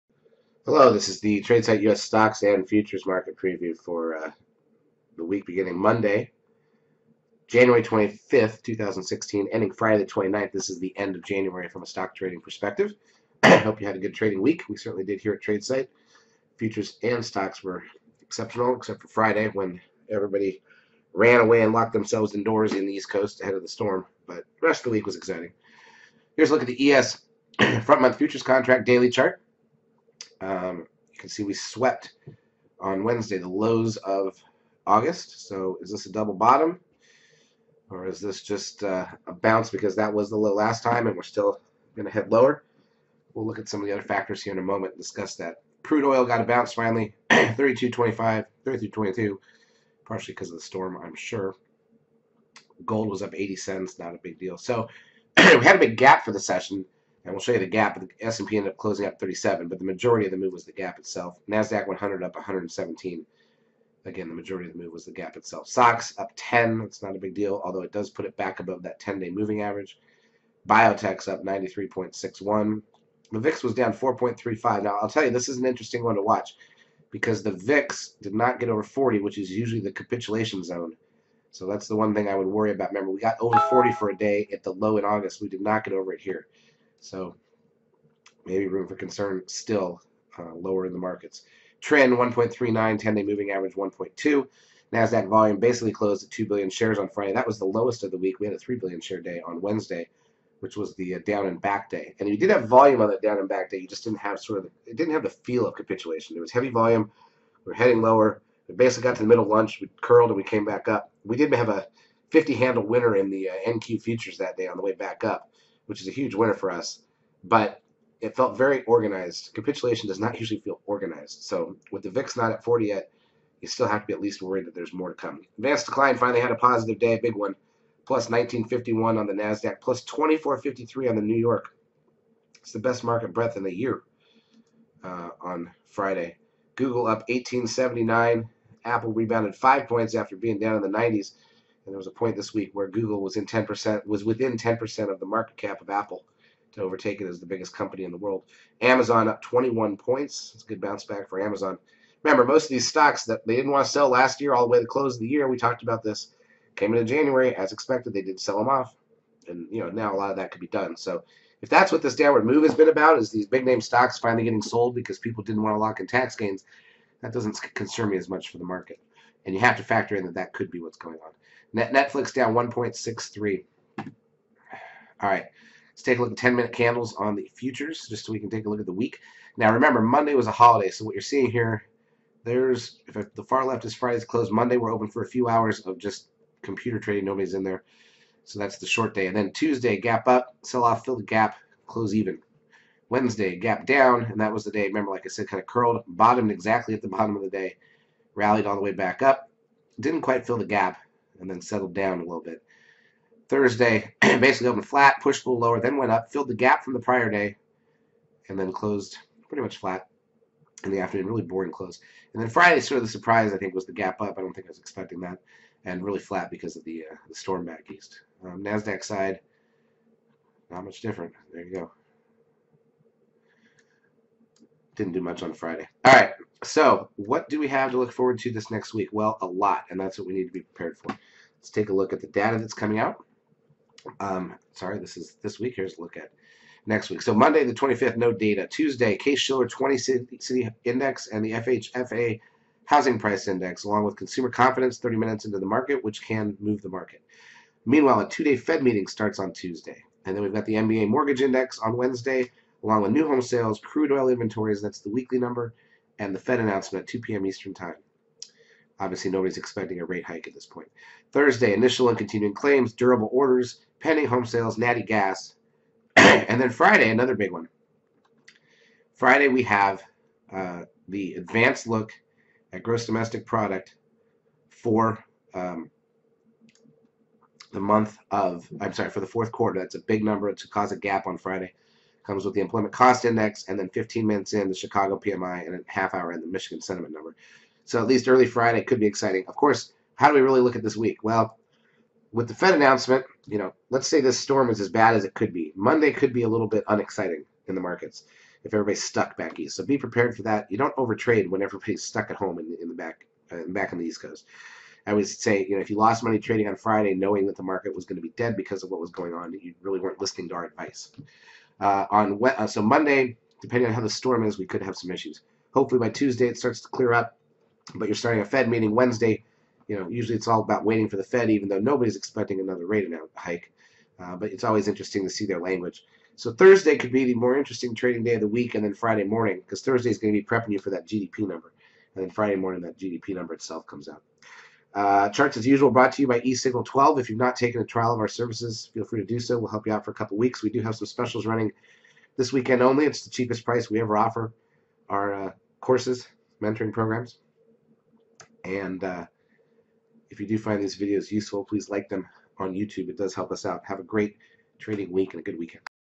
Stock Trading: Economic Roadmap and Index Preview for the Week of January 25, 2016
This video will give you preview of what is going on in the US stocks and futures markets and what key economic data to focus on in the week ahead.